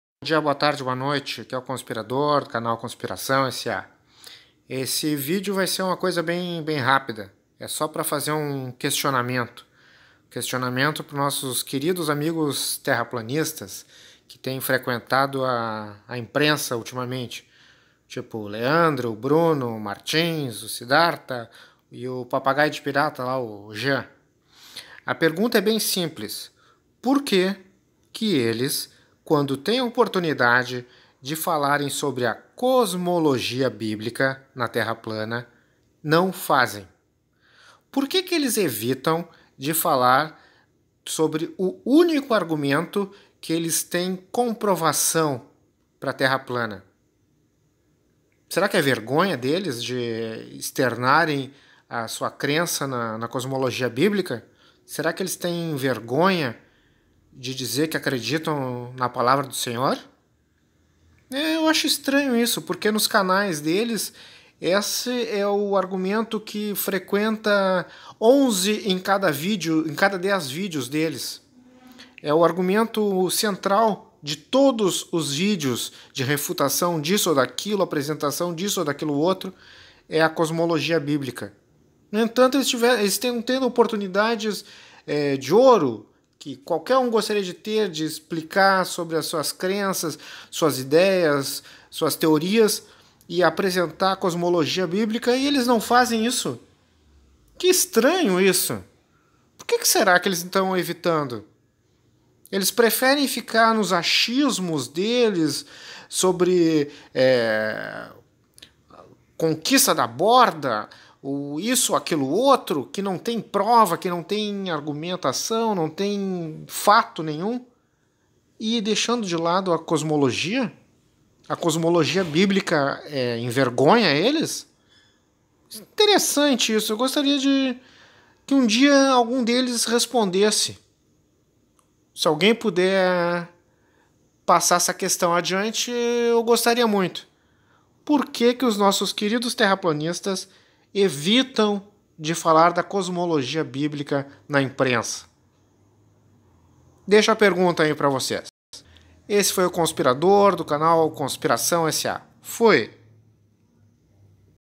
TERRAPLANISTAS COM VERGONHA DA COSMOLOGIA BÍBLICA?
Qual o motivo da Cosmologia Biblica não ser apresentada e defendida na imprensa pelos representantes terraplanistas? Qual a razão do silêncio sobre o assunto?